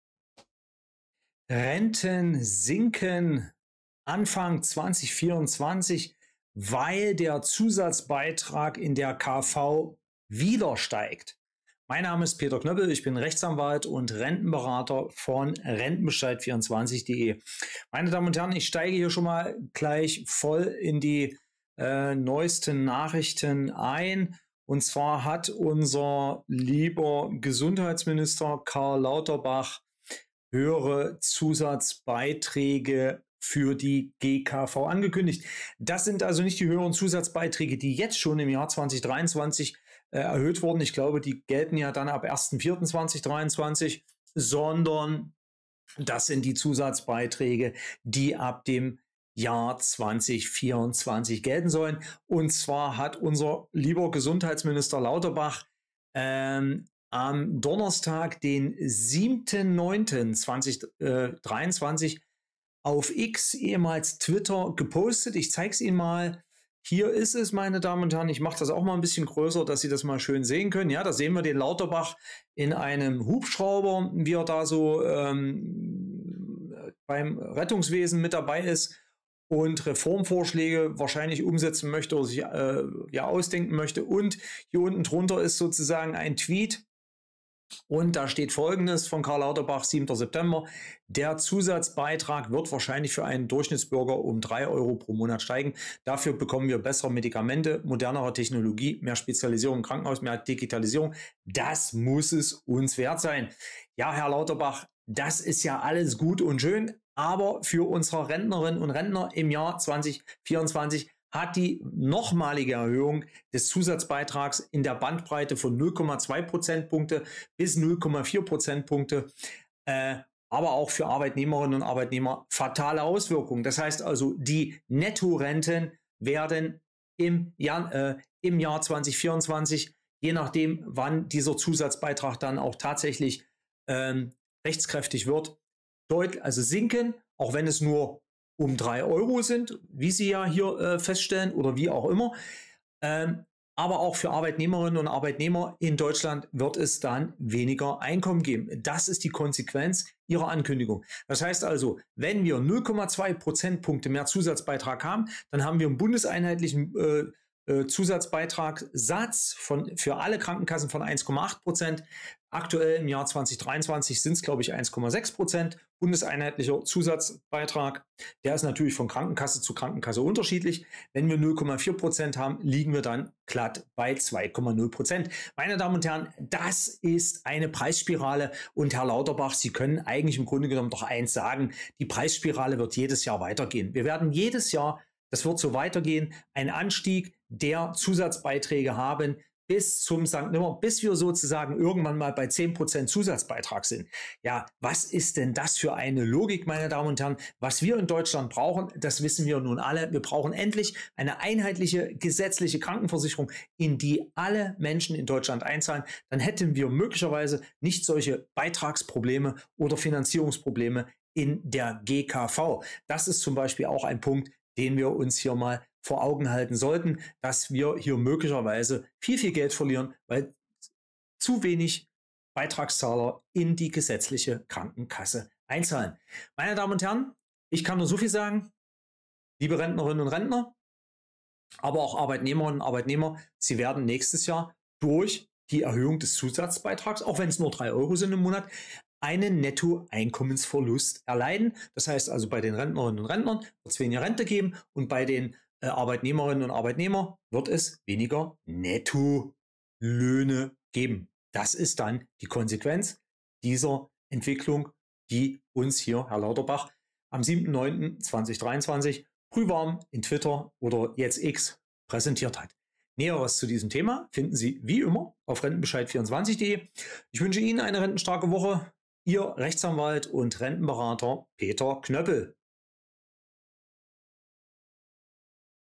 2024: Nettorenten-Verlust für Millionen Rentner. Zusatzbeitrag in der gKV steigt wieder!
Weniger Nettorente für Millionen Rentnerinnen und Rentner. Zusatzbeitrag in der gesetzlichen KV soll wieder um 0,2 bis 0,4 % steigen. Es fehlen rund 7 Milliarden Euro an Einnahmen. Erhöht sich der Zusatzbeitrag sinkt deshalb rechnerisch die Netto-Rente - Auszahlbetrag. Denn der Rentner muss mehr Beitrag in der GKV als Zusatzbeitrag zahlen.